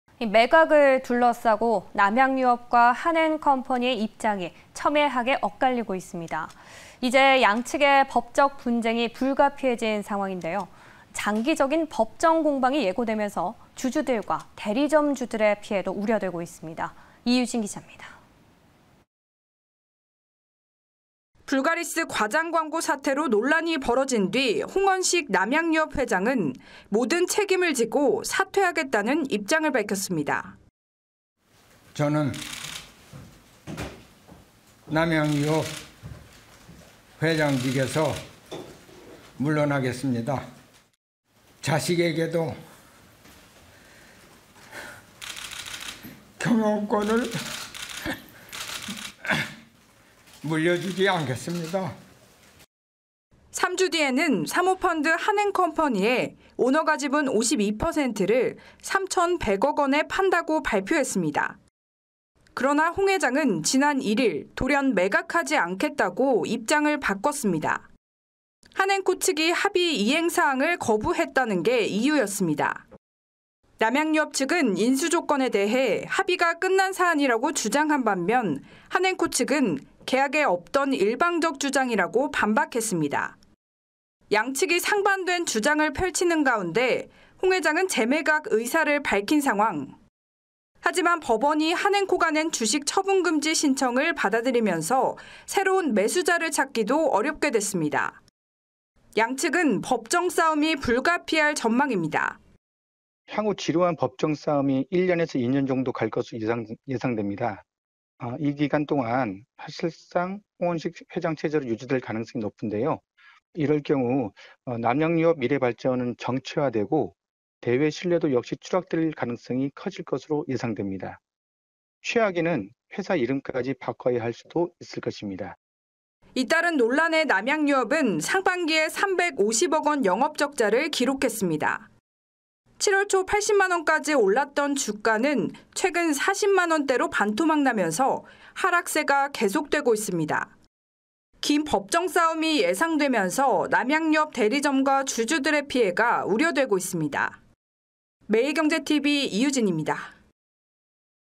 [매일경제TV 뉴스] 남양유업 매각 '안갯속'…"주주·점주 피해우려"
【 앵커멘트 】
매각을 둘러싼 남양유업과 한앤컴퍼니의 입장이 첨예하게 엇갈리고 있습니다.

이제 양측의 법적 분쟁이 불가피해진 상황인데요.

장기적인 법정 공방이 예고되면서, 주주들과 대리점주들의 피해도 우려되고 있습니다.

이유진 기자입니다.

【 기자 】
불가리스 과장광고 사태로 논란이 벌어진 뒤, 홍원식 남양유업 회장은 모든 책임을 지고 사퇴하겠다는 입장을 밝혔습니다.

▶ 인터뷰 : 홍원식 / 남양유업 회장
- "저는 남양유업 회장직에서 물러나겠습니다. 또한 자식에게도 경영권을 물려주지 않겠습니다 "

「3주 뒤에는 사모펀드 한앤컴퍼니에 오너가 지분 52%를 3천100억원에 판다고 발표했습니다.

그러나 홍 회장은 지난 1일, 돌연 매각하지 않겠다고 입장을 바꿨습니다.

한앤코 측이 합의 이행사항을 거부했다는 게 이유였습니다.  」

남양유업 측은 인수 조건에 대해 "합의가 끝난 사안"이라고 주장한 반면 한앤코 측은 "계약에 없던 일방적 주장"이라고 반박했습니다.

양측이 상반된 주장을 펼치는 가운데 홍 회장은 재매각 의사를 밝힌 상황.

하지만 법원이 한앤코가 낸 주식처분금지 신청을 받아들이면서 새로운 매수자를 찾기도 어렵게 됐습니다.

양측은 법정 싸움이 불가피할 전망입니다.

▶ 인터뷰(☎) : 오일선 / 한국CXO연구소장
- "향후 법정 싸움이 1년에서 2년 정도 될 것으로 예상되는데요.

이 기간 동안 사실상 홍원식 회장 체제로 유지될 가능성이 높은데 이럴 경우 남양유업 미래 발전은 정체화되고 대외 신뢰도 역시 추락할 가능성이 커질 것으로 예상됩니다. 최악에는 회사 이름까지 바꿔야 할 수도 있을 것입니다."

잇따른 논란의 남양유업은 상반기에 350억원 영업적자를 기록했습니다.

7월 초 80만원까지 올랐던 주가는 최근 40만원대로 반토막나면서 하락세가 계속되고 있습니다.

긴 법정 싸움이 예상되면서 남양유업 대리점과 낙농가, 주주들의 피해가 우려되고 있습니다.